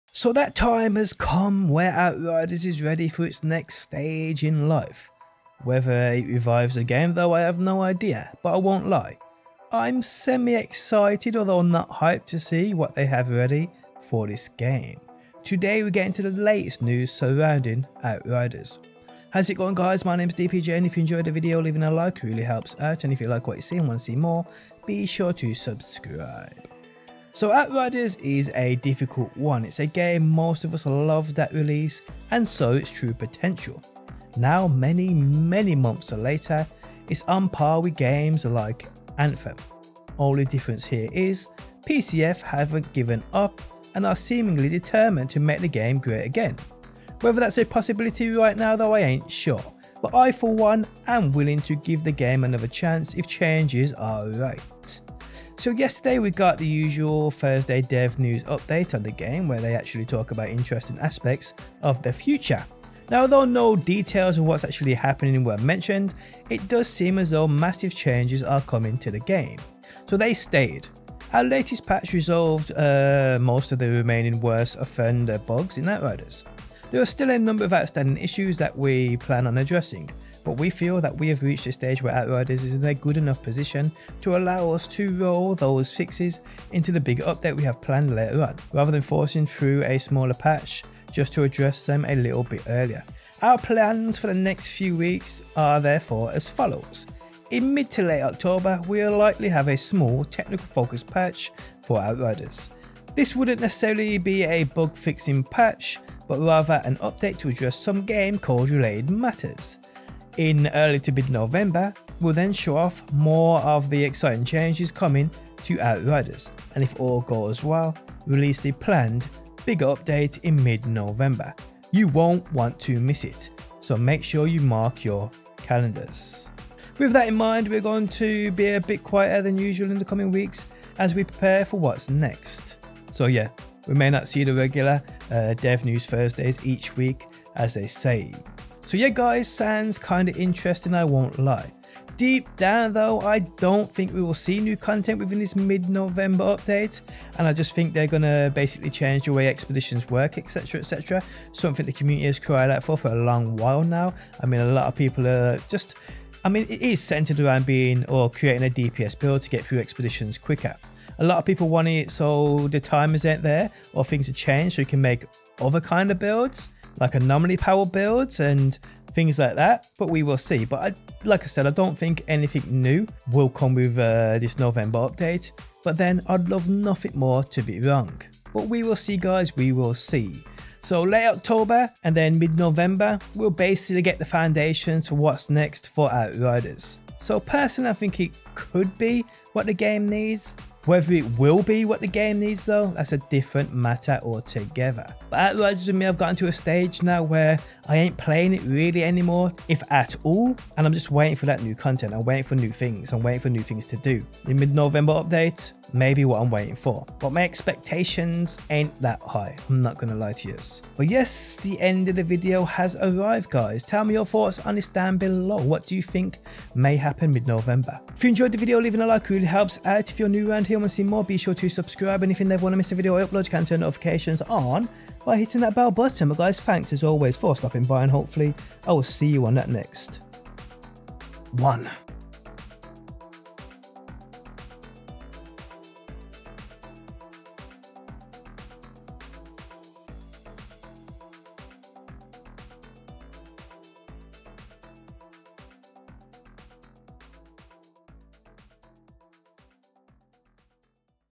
Outriders: Will This Save The Game? - MAJOR NEW UPDATE COMING - DLC / NEW CONTENT Before X-Mas?
A major game chaniging Outriders patch is coming, will it save the game?